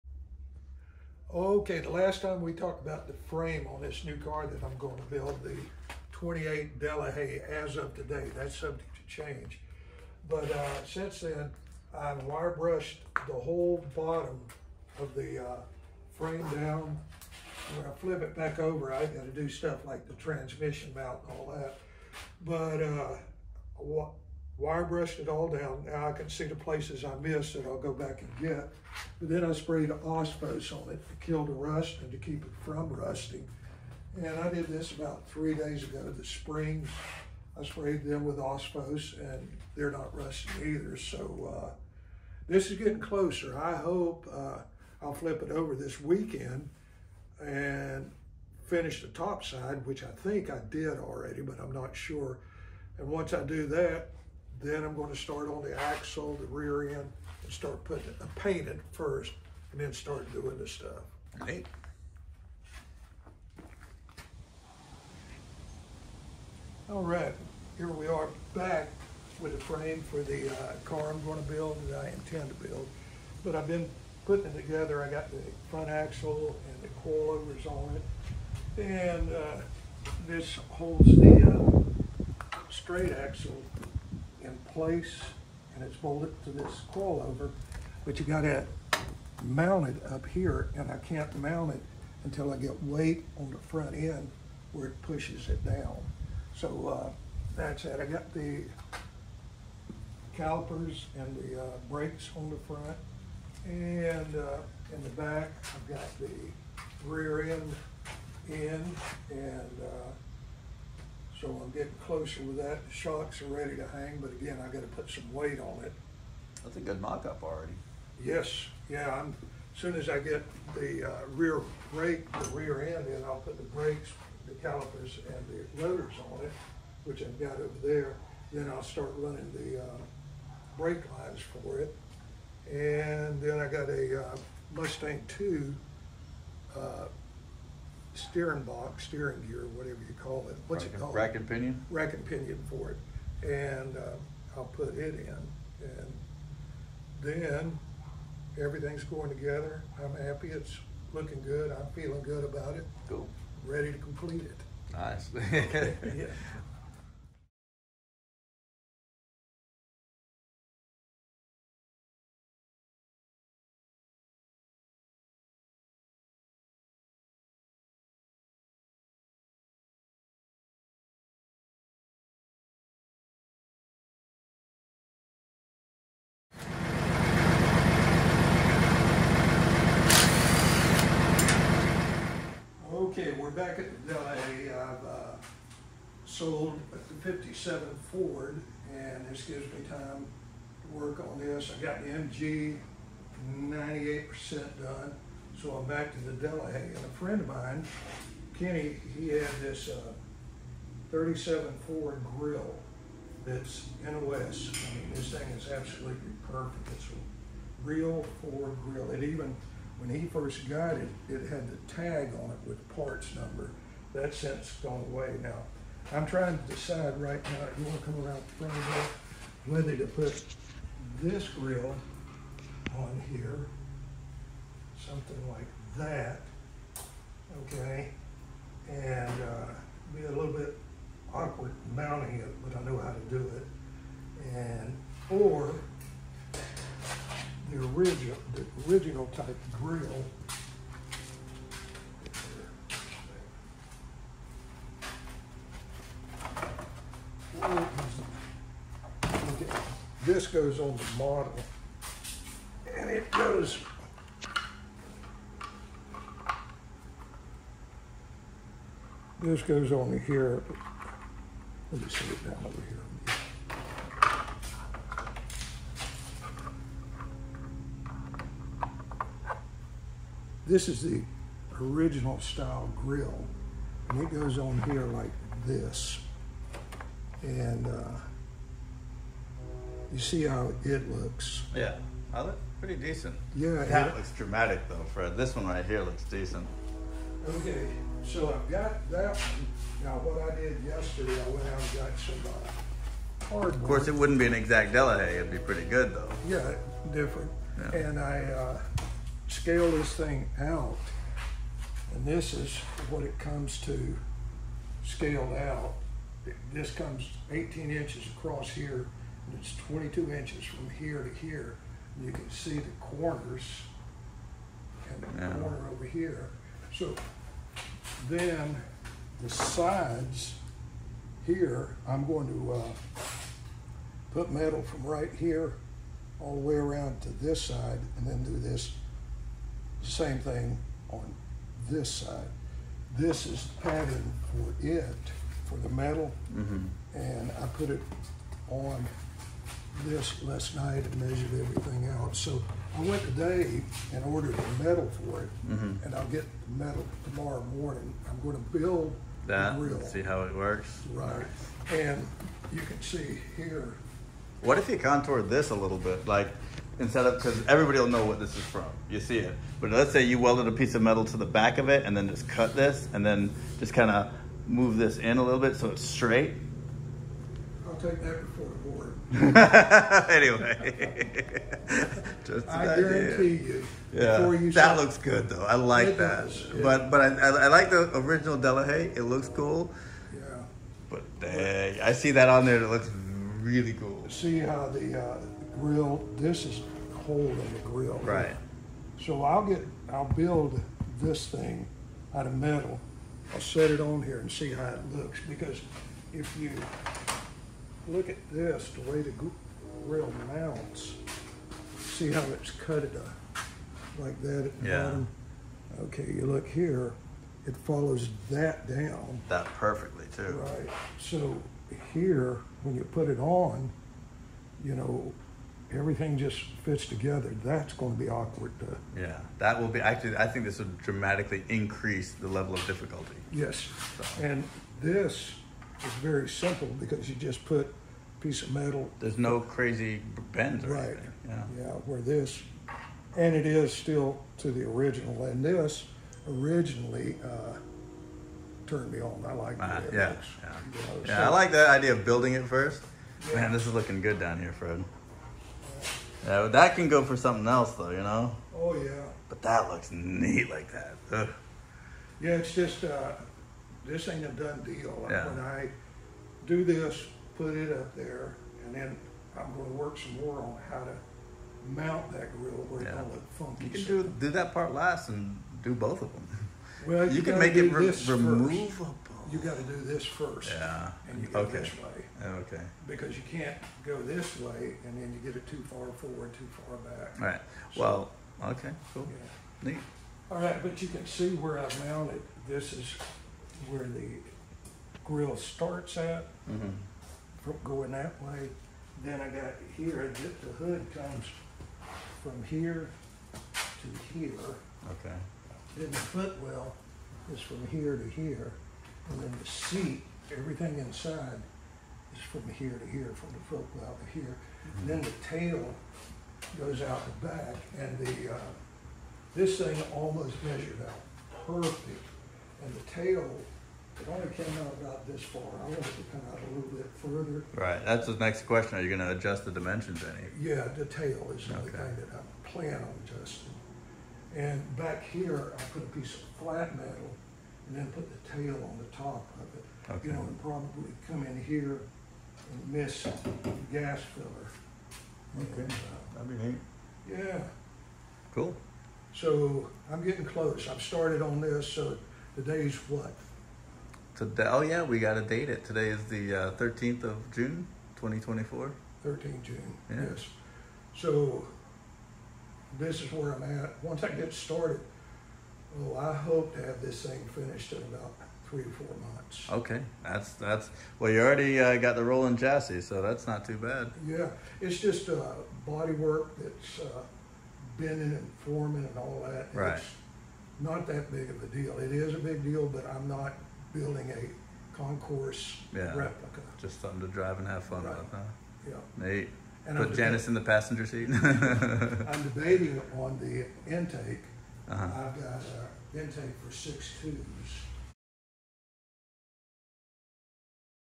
1926 Delahaye 15 s8 pt. 1
As usual, I show Fred a beautiful car and he says I think I can build that. So now we start building this 1926 race car. Starting off with a frame prebuilt this time from a Chevrolet using a Chevrolet and Ford Drive train the rest is all fabrication. Enjoy.